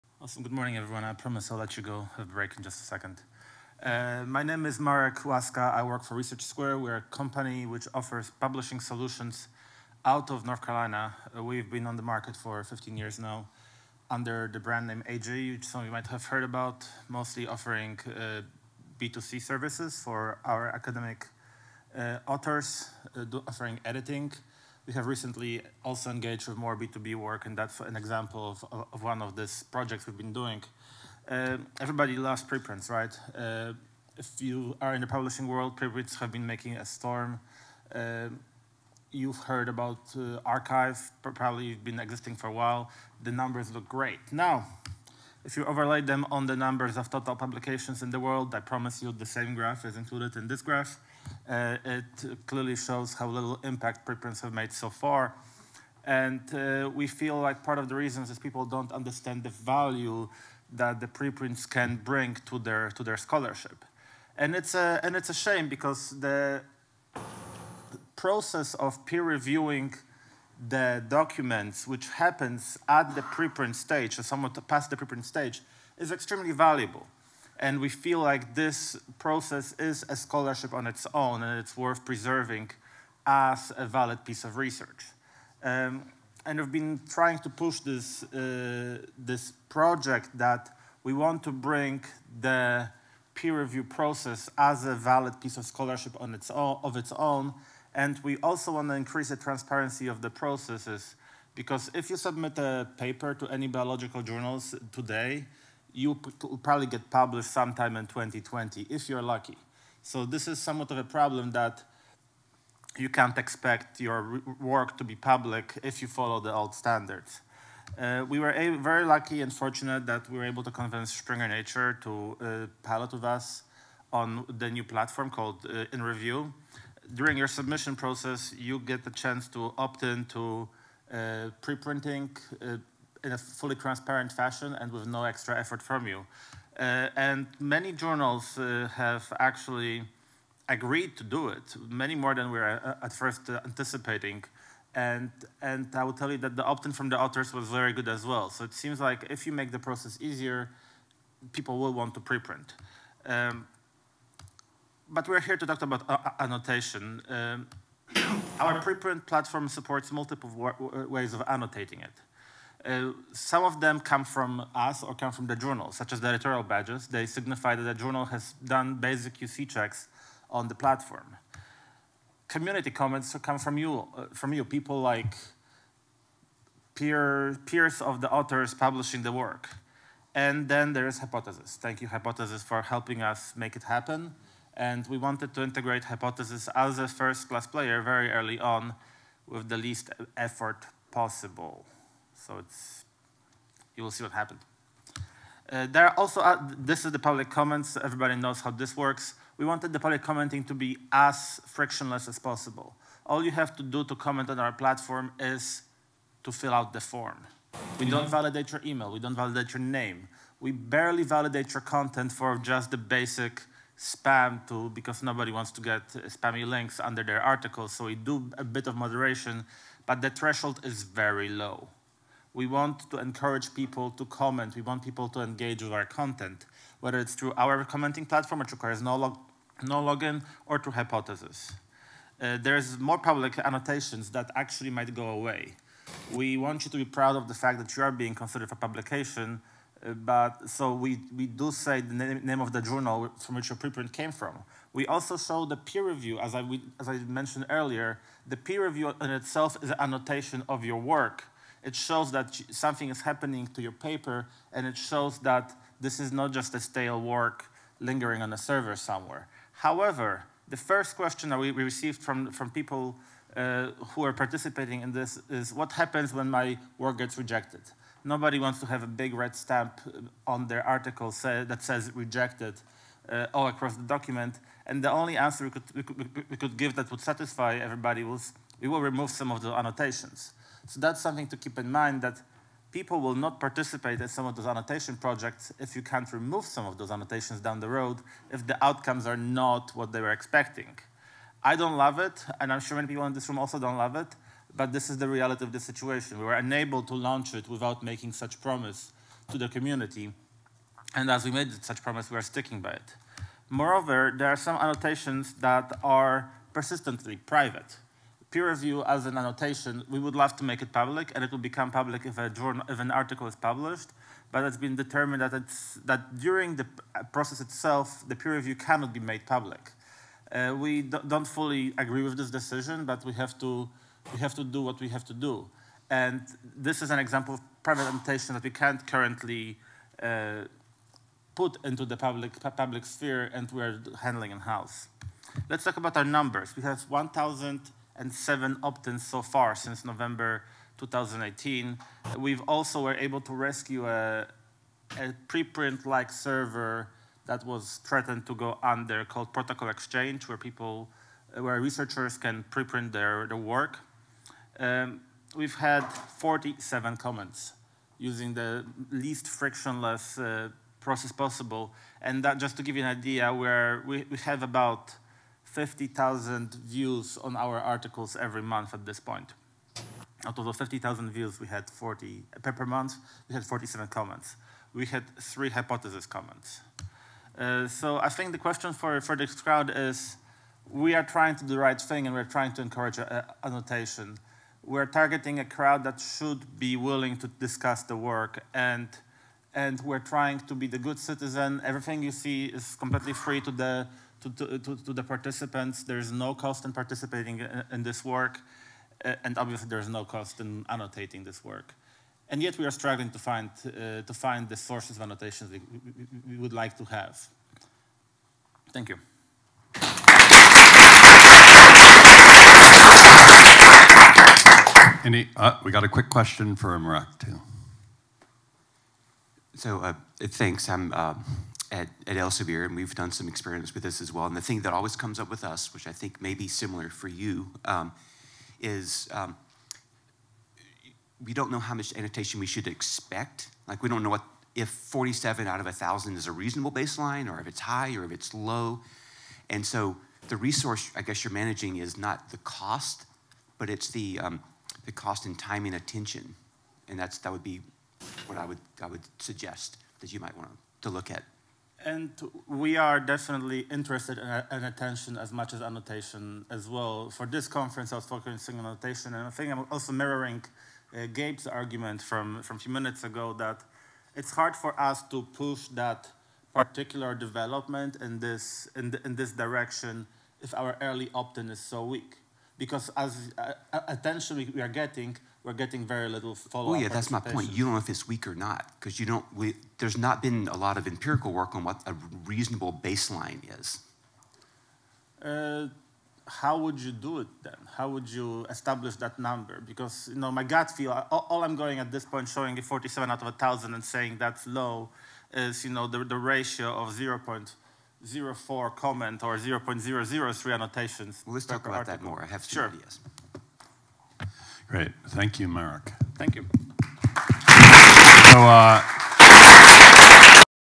ianno19 Flash Talk: Marek Laska, Research Square
Marek Laska of Research Square presenting at the seventh annual I Annotate gathering in Washington DC. Presentation slides Explore